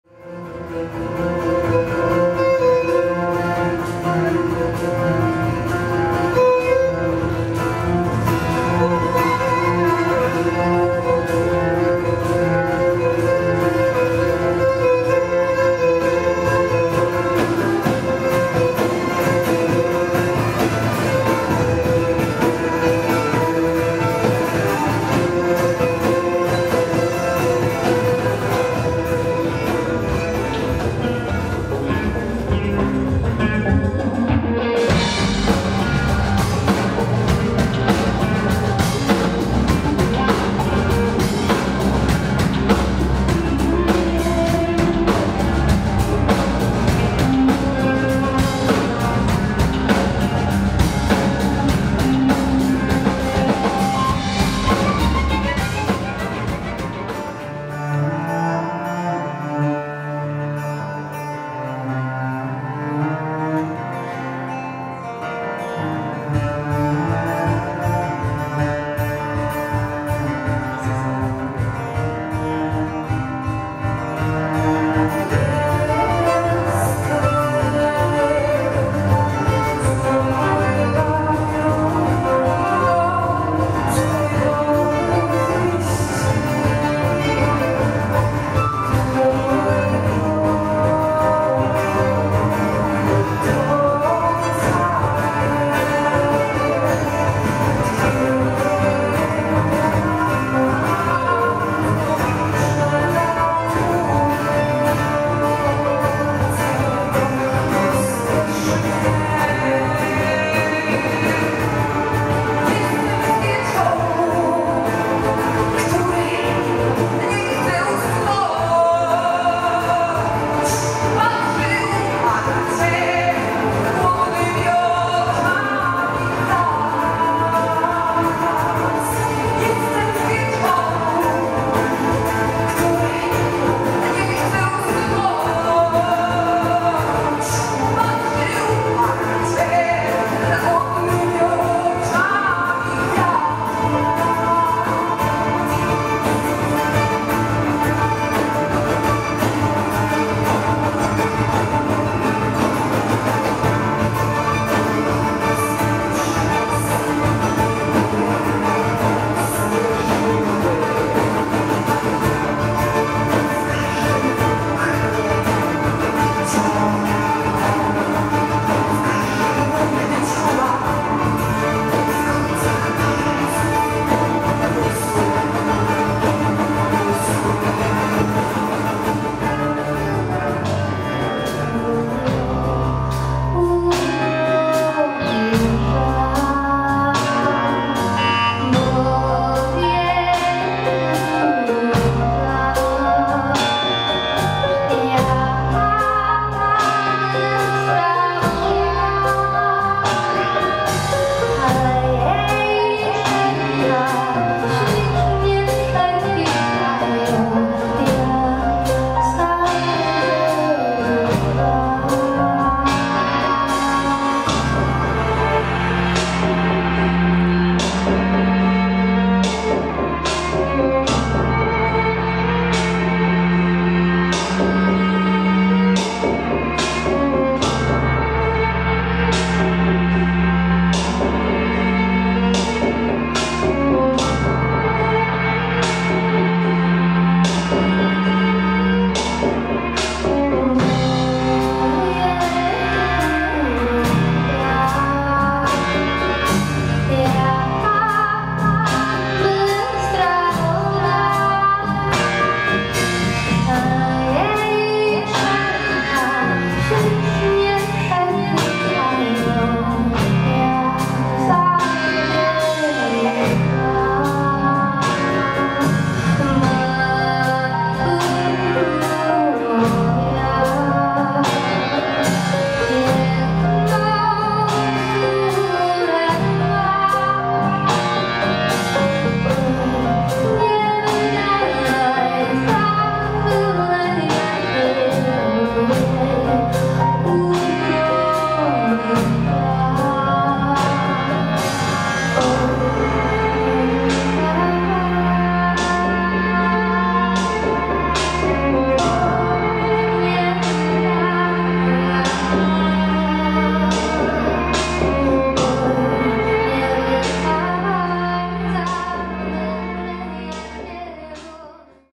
DUCH & Digit All Love
Moim zamierzeniem nie było porównywanie obu zespołów, zafascynował mnie artykuł tam pełny zapis, oba zespoły znam i podziwiam za to co tworzą, muzyka obu tych zespołów wymyka się z zaszufladkowania, to muzyka jak jeden z moich rozmówców powiedział, muzyka na lepsze czasy.
jednym z moich marzeń to wspólny występ obu formacji i co najmniej jeden utwór wspólnie zagrany...
F0800